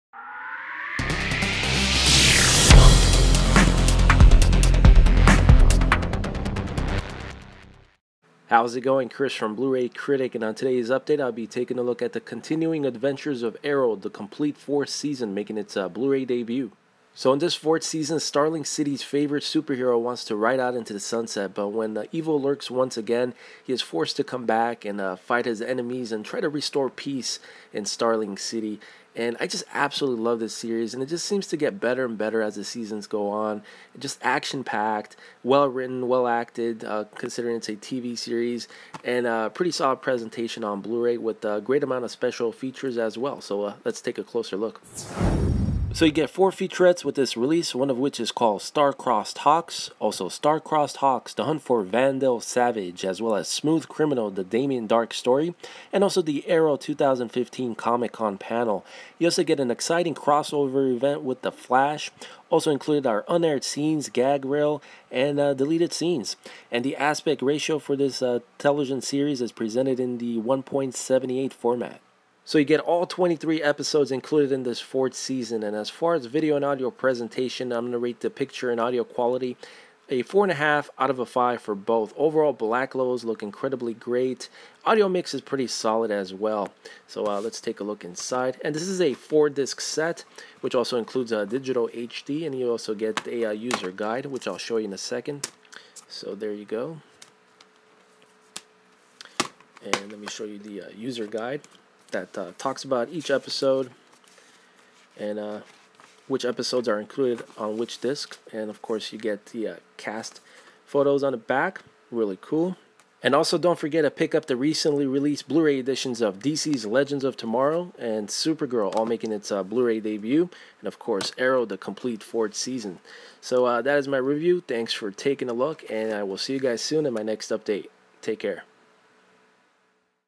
Arrow: The Complete Fourth Season Blu Ray Unboxing and Review
Unboxing and reviewing Arrow: The Complete Fourth Season on Blu-ray, released by Warner Bros.

“Warner Bros. Home Entertainment provided me with a free copy of the DVD I reviewed in this Blog Post. The opinions I share are my own.”

Arrow: The Complete 4th Season (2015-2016) Blu-ray

This Blu-ray edition includes Digital HD. DVD edition is also available.

After defeating his most formidable foe to date and riding off into the sunset with longtime flame Felicity Smoak, Oliver Queen (aka The Arrow) left Star City with the hopes of beginning a new life. But will Oliver ever truly be able to leave behind his past as The Arrow, and, if so, what becomes of the team he has worked so hard to assemble? Will military vet John Diggle, Oliver’s sister Thea Queen, and lawyer-turned-vigilante Laurel Lance continue Oliver's fight without him? And with Malcolm Merlyn having ascended to the top of the League of Assassins as the new Ra's al Ghul, is anyone really safe?

With Blu-ray’s unsurpassed picture and sound, Arrow: The Complete Fourth Season Blu-ray release will include 1080p Full HD Video with DTS-HD Master Audio for English 5.1. The 4-disc Blu-ray will feature a high-definition Blu-ray and a Digital HD copy of all 23 episodes from Season Four, as well as The Flash crossover episode.

Arrow stars Stephen Amell (Private Practice), Katie Cassidy (Gossip Girl, Melrose Place), David Ramsey (Blue Bloods, Dexter), Willa Holland (The O.C.) and Emily Bett Rickards (Brooklyn), with John Barrowman (Desperate Housewives, Doctor Who), and Paul Blackthorne (The River).  Based on the characters from DC Comics, Arrow is produced by Bonanza Productions Inc. in association with Berlanti Productions and Warner Bros. Television, with executive producers Greg Berlanti (The Flash, Supergirl, Blindspot), Marc Guggenheim (Eli Stone), Andrew Kreisberg (The Flash, Supergirl), Wendy Mericle (Desperate Housewives) and Sarah Schechter (The Flash, Supergirl).

SPECIAL FEATURES:

Star Crossed Hawks featurette
Star Crossed Hawks: The Hunt for Vandal Savage featurette
Smooth Criminal: The Damien Darhk Story featurette
Arrow: 2015 Comic-Con Panel
Deleted Scenes
Gag Reel

23 ONE-HOUR EPISODES, PLUS THE FLASH CROSSOVER

Green Arrow
The Candidate
Restoration
Beyond Redemption
Haunted
Lost Souls
Brotherhood
8a. Legends of Today (The Flash; 1st part of 2-hour crossover event)
8b. Legends of Yesterday (Arrow; 2nd  part of 2-hour crossover event)
Dark Waters
Blood Debts
A.W.O.L.
Unchained
Sins of the Father
Code of Silence
Taken
Broken Hearts
Beacon of Hope
Eleven-Fifty-Nine
Canary Cry
Genesis
Monument Point
Lost in the Flood
Schism

Creators: Greg Berlanti, Marc Guggenheim, Andrew Kreisberg

Starring: Stephen Amell, David Ramsey, Willa Holland, Paul Blackthorne, Emily Bett Rickards, Katie Cassidy, John Barrowman, Colton Haynes, Susanna Thompson, Echo Kellum, Manu Bennett, Caity Lotz, Colin Donnell, Keri Adams, Celina Jade, Audrey Marie Anderson, Neal McDonough and Brandon Routh